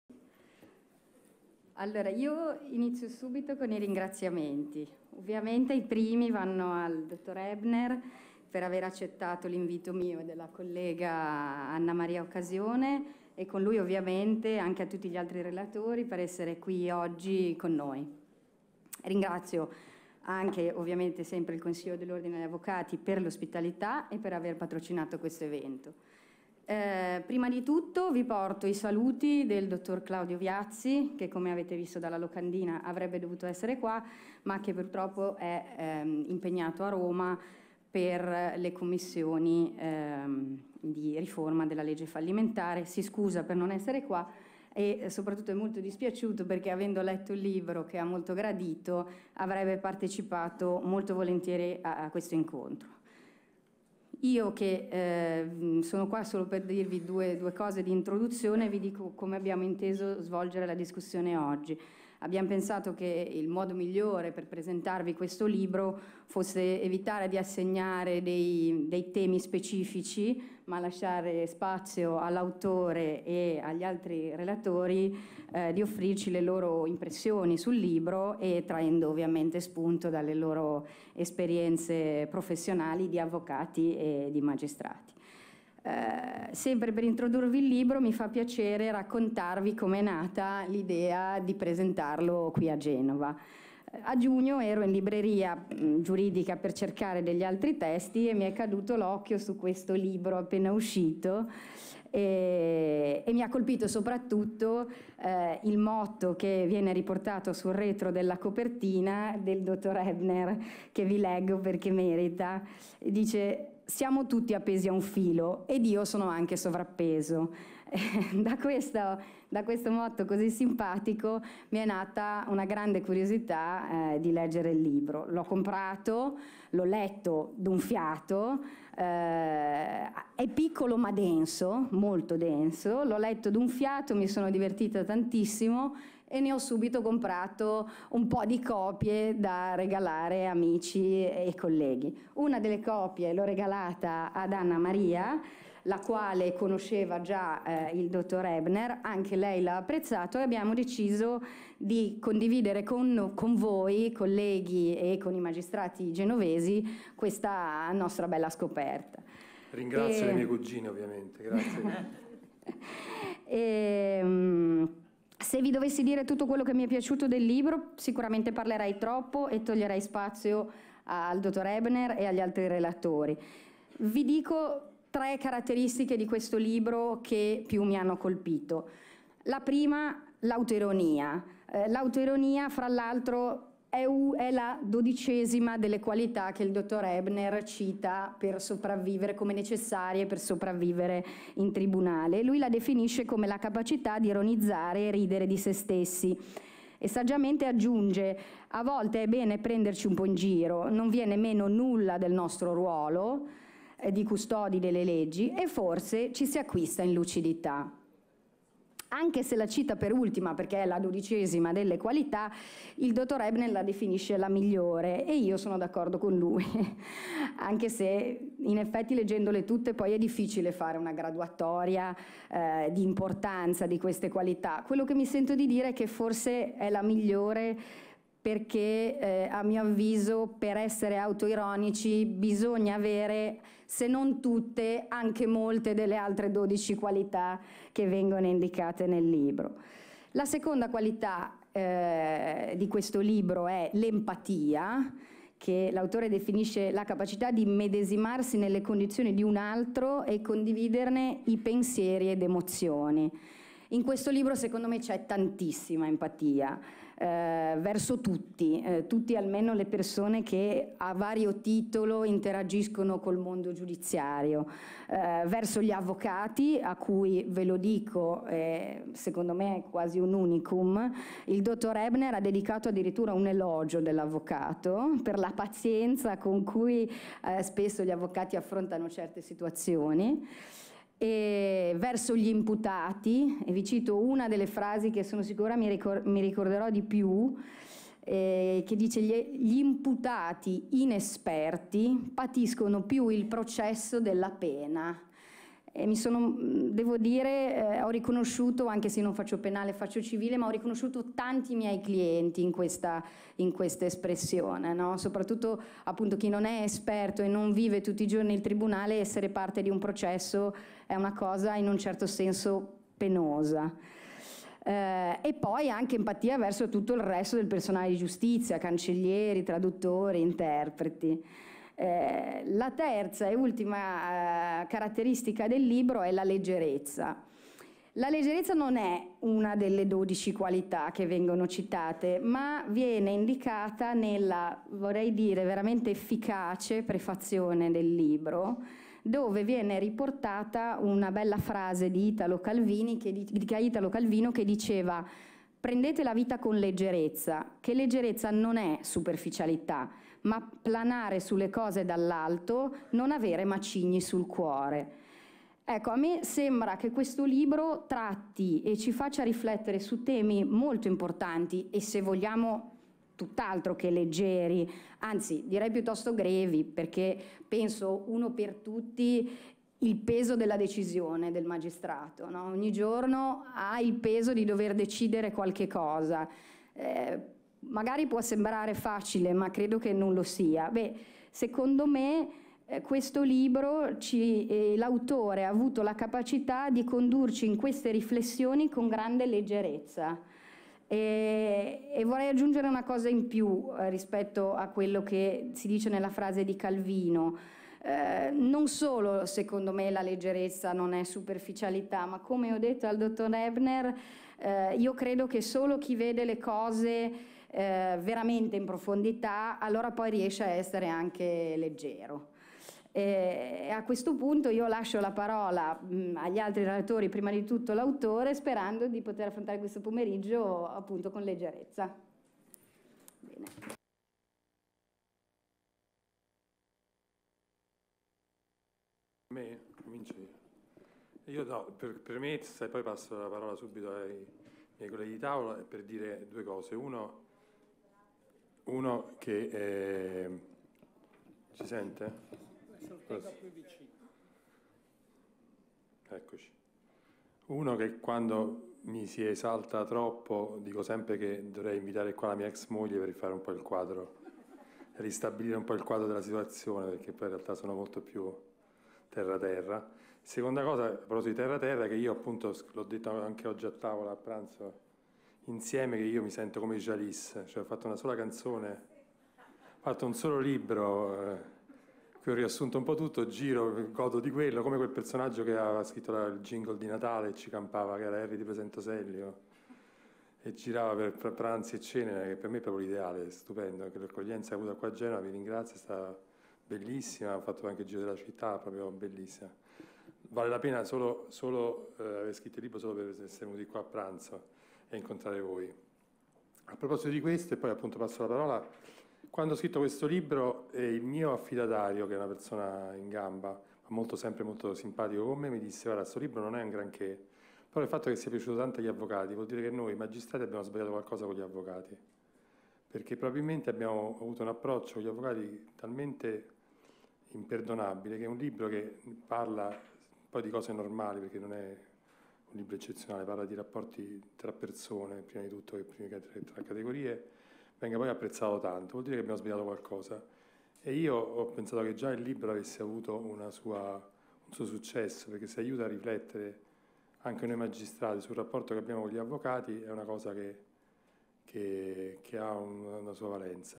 Presentazione del libro: “Dodici qualità per sopravvivere in tribunale (e non è nemmeno certo)”
Evento formativo F.P.C.
Organizzatore:  Consiglio dell'Ordine degli Avvocati di Genova
Luogo dell'evento: Genova - Via XII Ottobre, 3 - Centro Cultura Formazione e Attività Forensi
Data: 7 Dicembre 2017
Materia: Ordinamento Deontologico
Crediti materie deontologiche: 2
Durata: 01.24.59

Relatori:
Avv. Alessandro Vaccaro
Avv. Aurelio Di Rella Tomasi di Lampedusa
Dott.ssa Giovanna Dominici

aul@net: corsi e-Learning di aggiornamento F.P.C., realizzati e accreditati dall'Ordine degli Avvocati di Genova